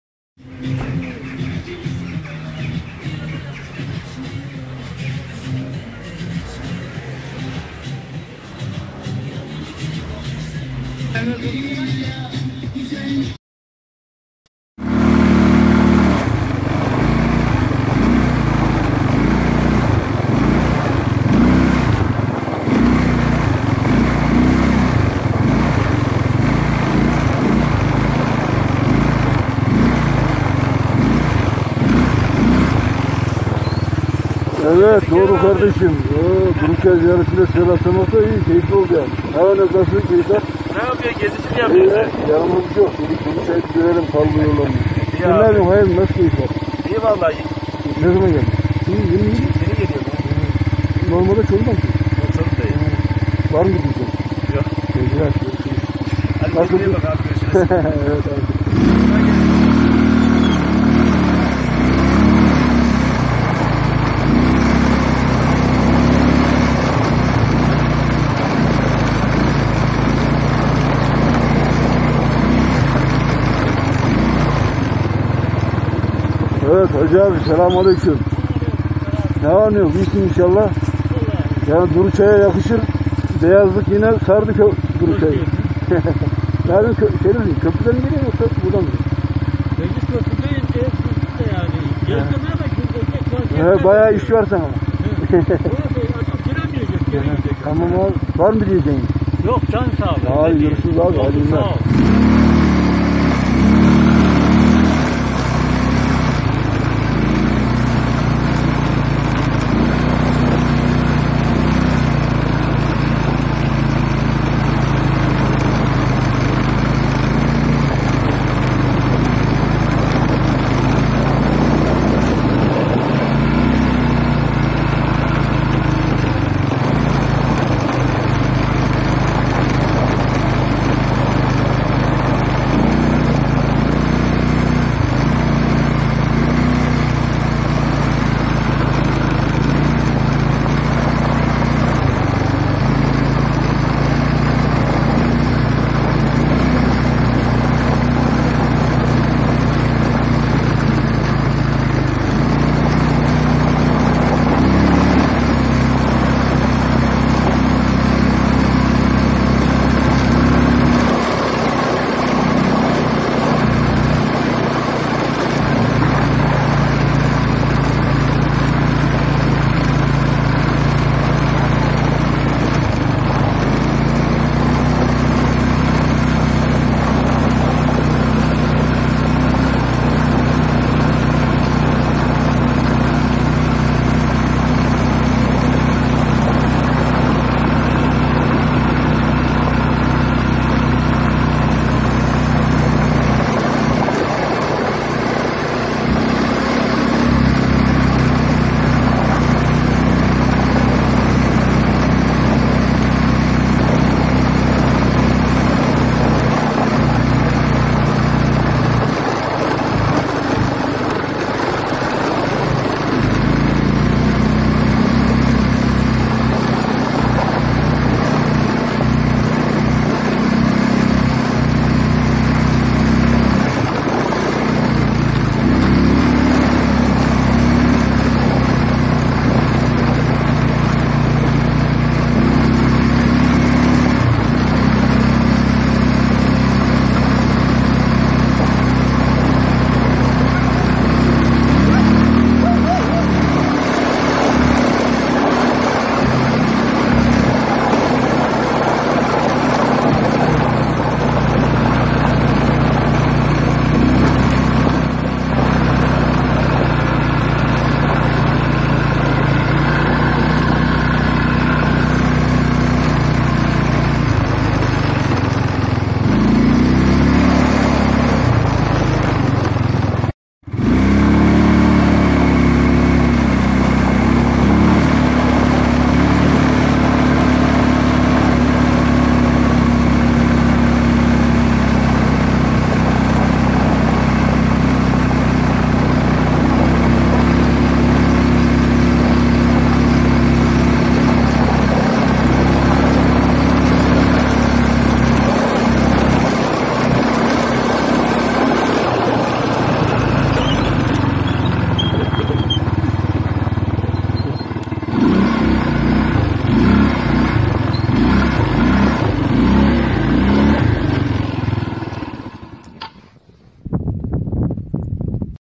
Duruçay Göleti buz tuttu. Ortaya müthiş görüntüler çıktı.
İşte çok özel görüntüler.
KAR İÇİNDE GÖMÜLEN DURUÇAY